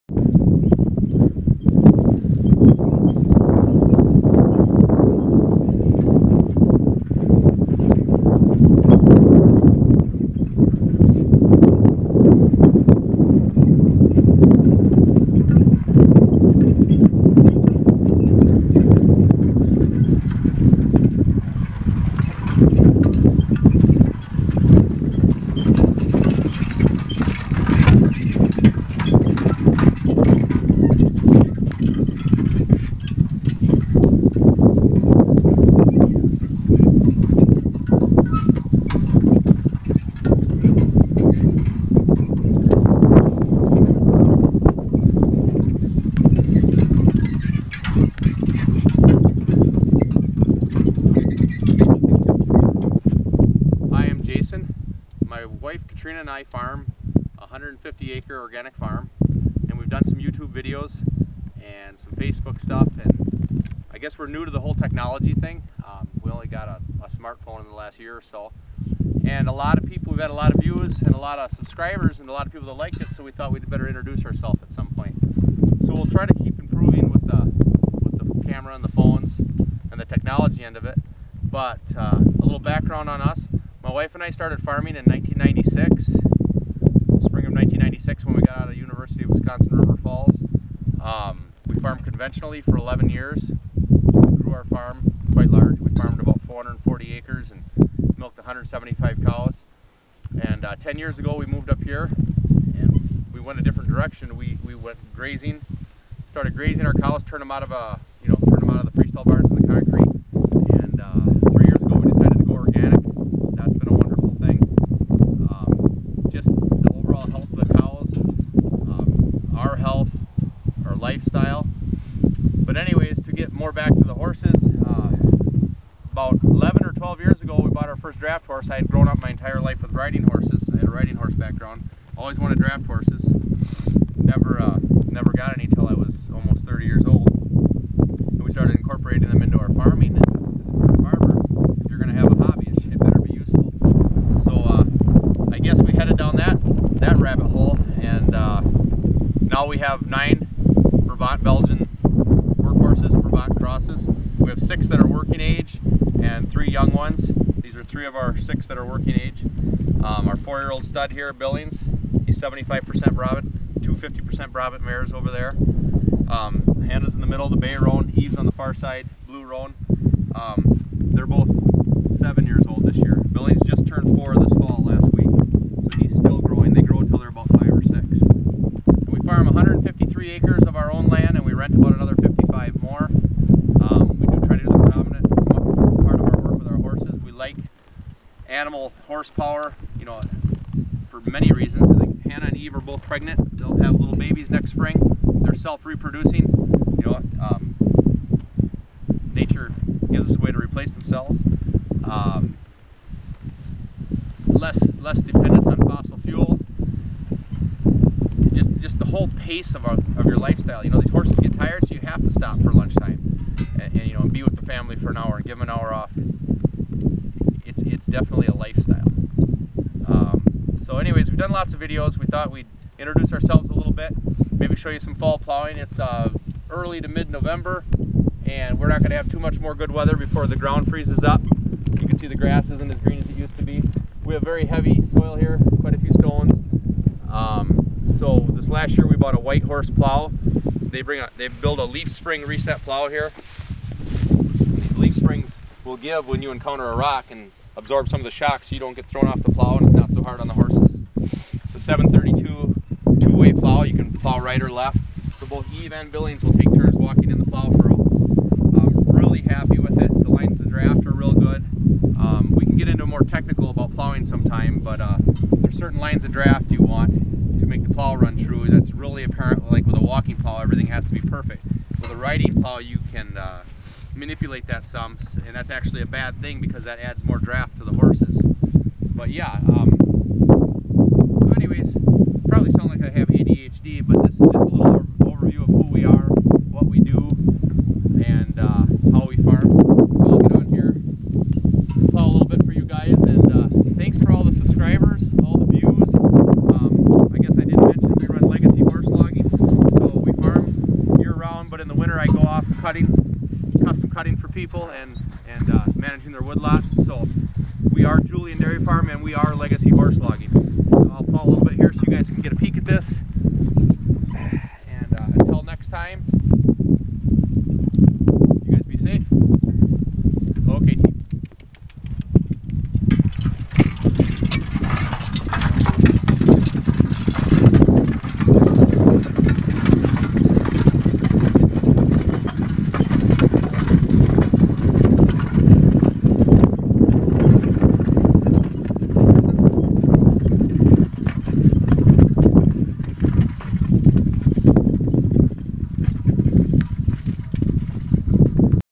Introducing Julian Dairy farm/ legacy horse logging
Brabant Horses plowing with Whitehorse plow